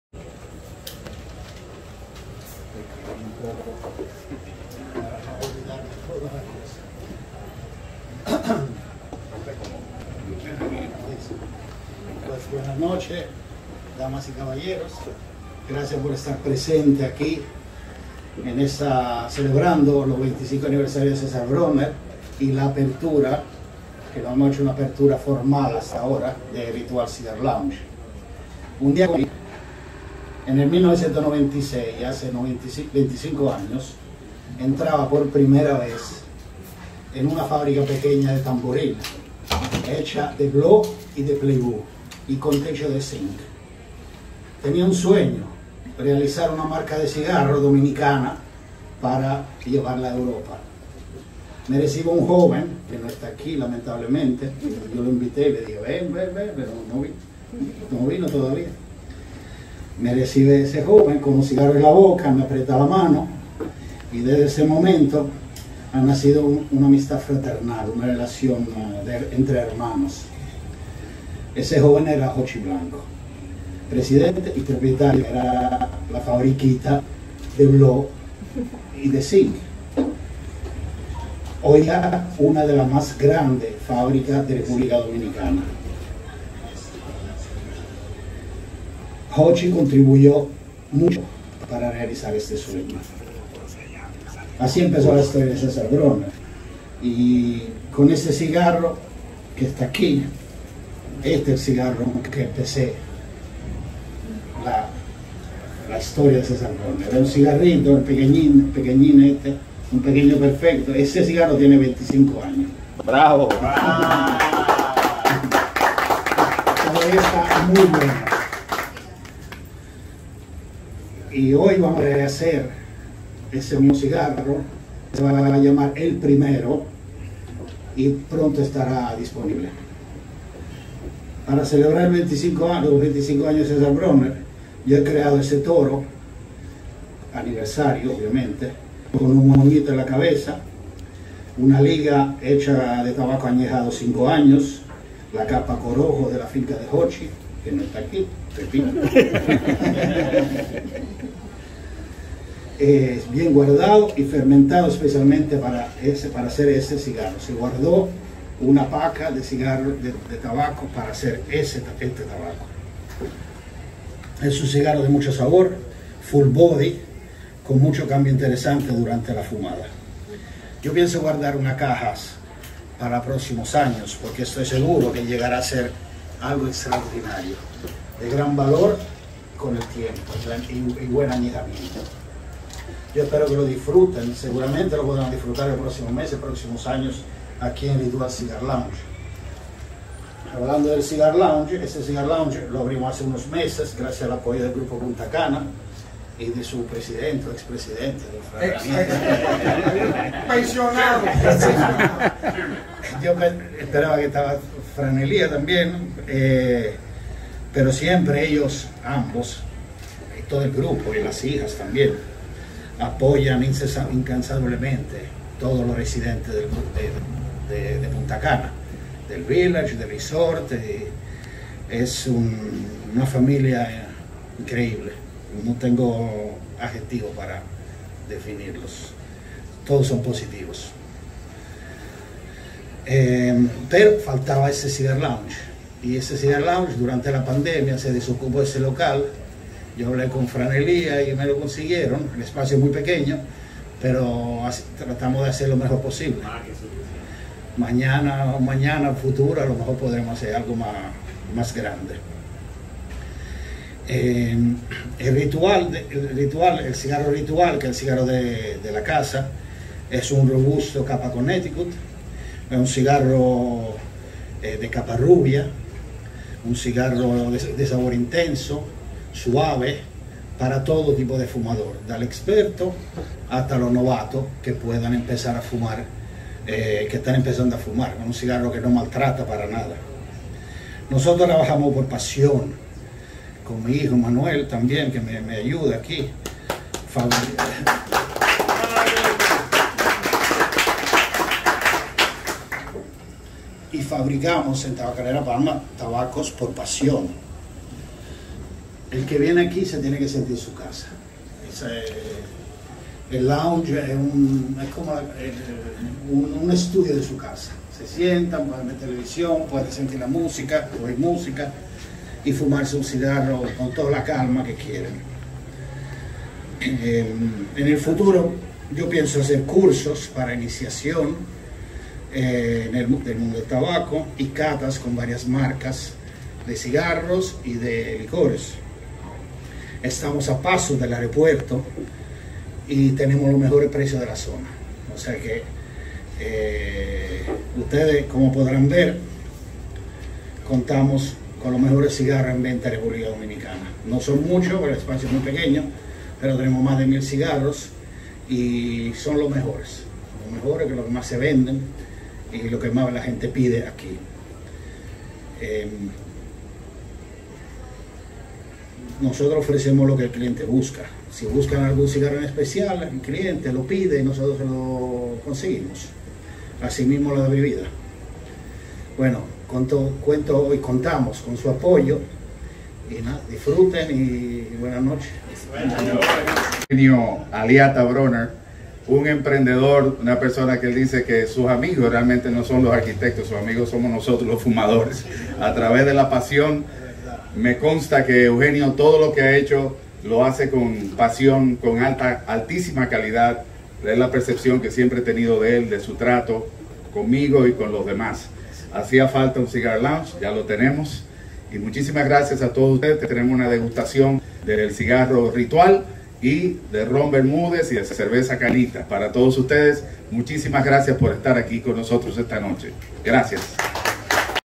Cigarro Dominicano, Eugenio Aliatta, Inauguracion Ritual & 25 Aniversario Cigarro Cezar Bronner
pueden ver y disfrutar de su lectura, En Español y en Inglés, de Este y Otros Reportajes e Informaciones de Su Interés

LA MARCA DE CIGARROS CEZAR BRONNER CELEBRA SU 25 ANIVERSARIO EN EL NUEVO RITUAL CIGAR LOUNGE, DE PUNTACANA VILLAGE

Fotos: Ritual Cigar Lounge / Fotógrafa: Gina Decamps

En un ambiente exquisito y con invitados de alto nivel, Eugenio Alliata Bronner celebró el vigésimo quinto aniversario de su marca de cigarros Cezar Bronner.

Durante el acto de celebración de los 25 años de la marca, su fundador y presidente, se dirigió a los presentes, resaltando las cualidades de sus cigarros, su compromiso con la calidad a través de todos estos años y presentó la nueva línea Cezar Bronner 25 Aniversario, lanzada para festejar esta ocasión tan especial.  Esta nueva línea se caracteriza por ser una fumada de mucho cuerpo, aroma exquisito y gran sabor, que deja un excelente gusto en el paladar.

En sus palabras, el Arq. Alliata recordó que produce la marca de cigarros Cezar Bronner desde el año 1996, en la fábrica Tabacalera Palma, de José -Jochy- Blanco, en Tamboril, la cual es hecha a mano con finos tabacos cosechados en República Dominicana, tabacos seleccionados, muy añejados y que cumplen con estrictos estándares de calidad.

Alliata finalizó sus palabras, expresando la satisfacción de haber establecido un nuevo espacio para disfrutar “como si estuviéramos en el salón de nuestra casa”.  Se trata de “Ritual Cigar Lounge”, un punto de encuentro para un exigente grupo de fumadores y el lugar estratégico para la venta y distribución de cigarros de alta calidad. “Ritual Cigar Lounge” es dirigido por Manuel Alliata, quien tiene a su cargo ofrecer no solo una fina selección de grandes marcas de cigarros, sino también una amplia selección de licores, destilados y cócteles propios del bar.  Este nuevo cigar lounge está ubicado en Galería Comerciales de Puntacana Village y abre todos los días, desde la 11 de la mañana.

THE CEZAR BRONNER CIGAR BRAND CELEBRATES ITS 25TH ANNIVERSARY AT THE NEW RITUAL CIGAR LOUNGE, IN PUNTACANA VILLAGE

Photos: Ritual Cigar Lounge / Photographer: Gina Decamps

In a cozy atmosphere and with high-level guests, Eugenio Alliata Bronner celebrated the twenty-fifth anniversary of his cigar brand Cezar Bronner.

During the brand 25 years celebration, its founder and president, addressed those present, highlighting the cigar qualities, his commitment to quality throughout all these years and presented the new Cezar Bronner 25 Anniversary line, launched to celebrate this very special occasion. This new line is characterized by being a full-bodied smoke, exquisite aroma and great flavor, which leaves an excellent taste on the palate.

In his speech, Alliata recalled that he has been producing the Cezar Bronner cigar brand since 1996, at Tabacalera Palma cigar factory, owned by José -Jochy- Blanco, in Tamboril, which is handmade with fine tobaccos harvested in the Dominican Republic, selected cigars, very aged and that meet strict quality standards.

Alliata finished his words, expressing the satisfaction of having established a new space to enjoy "as if we were in the living room of our house". It is the “Ritual Cigar Lounge”, a meeting point for a demanding group of smokers and the strategic place for the sale and distribution of high-quality cigars. “Ritual Cigar Lounge” is managed by Manuel Alliata, who is in charge of offering not only a fine selection of great cigar brands, but also a wide selection of spirits and typical cocktails of the bar. This new cigar lounge is located in the Commercial Gallery of Puntacana Village and is open every day, from 11 in the morning.